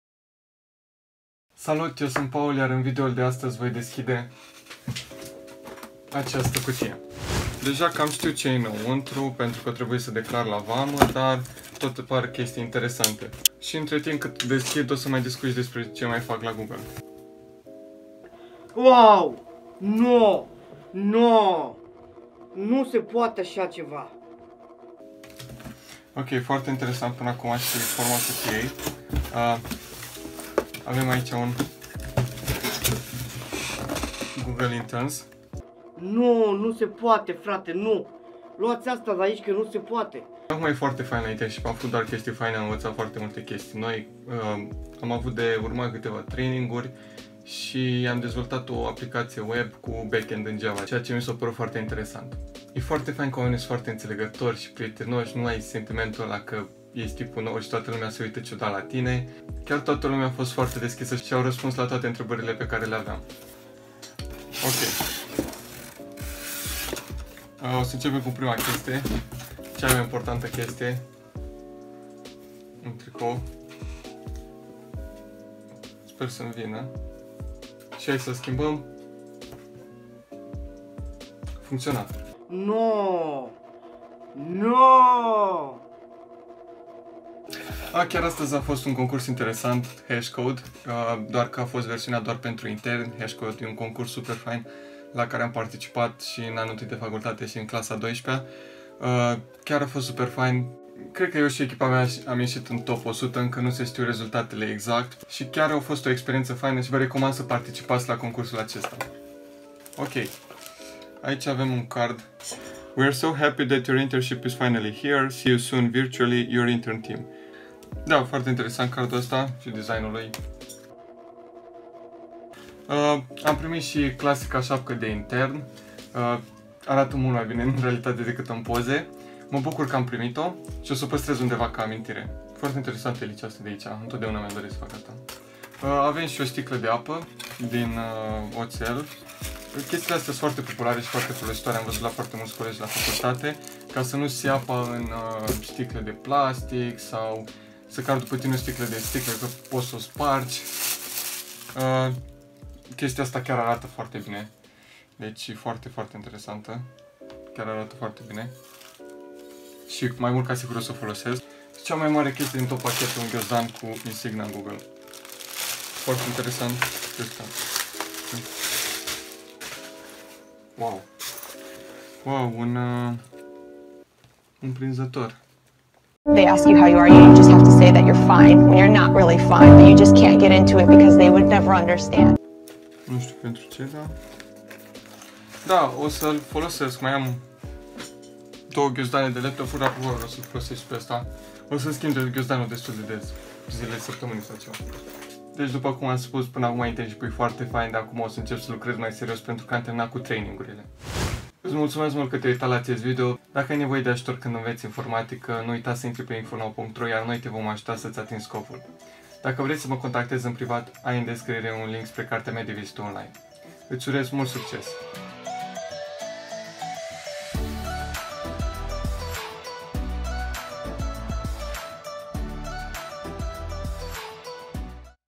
Ce am primit în cadoul de la Google?
În videoul de astăzi am deschis cadoul de la Google, pe care îl primesc internii.

👩‍💻Dacă vrei să înveți informatică de la persoane 🅱🅸🅽🅴 pregătite sau vrei la rândul tău să 🅰🅹🆄🆃🅸 alte persoane dornice să invețe programare, intră aici